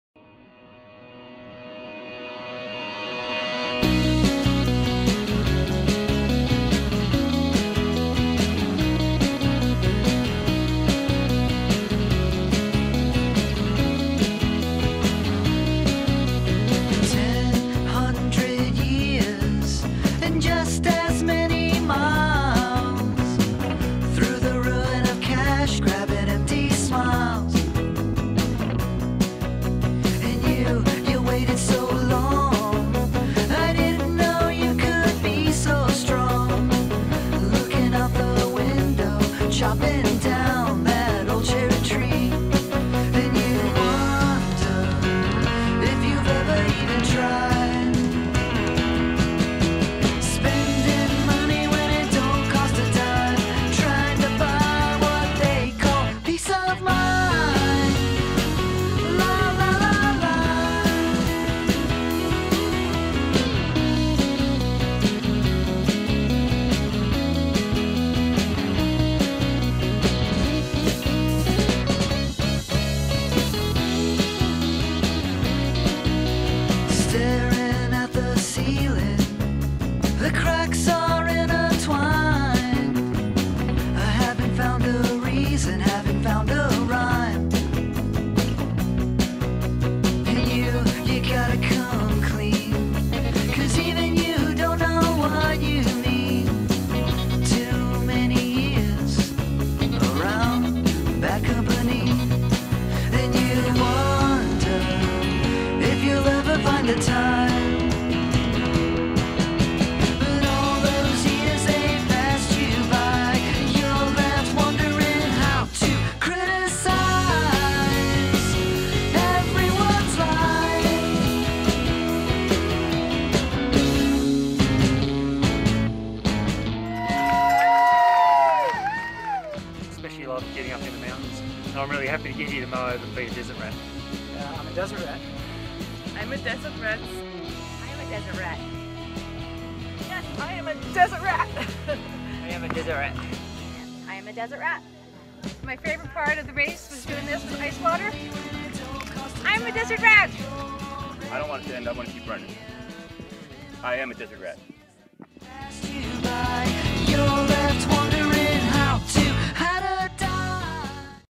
Desert RATS Kokopelli 150 2018
Unique beauty, amazing wildlife and profound solitude – all the elements of true adventure – reward racers willing to brave the strenuous Desert RATS journey. Vast landscape and ancient obstacles combine to create the rugged and stunning beauty of the Kokopelli Trail. Desert RATS is more than just a race. It is not only a journey through true backcountry desert but within yourself.  While some will race, most are there to test their mental and physical limits. Don’t expect to set personal records for the distance you’ll cover, you will need to pace yourself for the heat and terrain. You must have patience and grit in order to not be fighting your own frustration. The race is about the experience, pushing yourself and making friends along the way.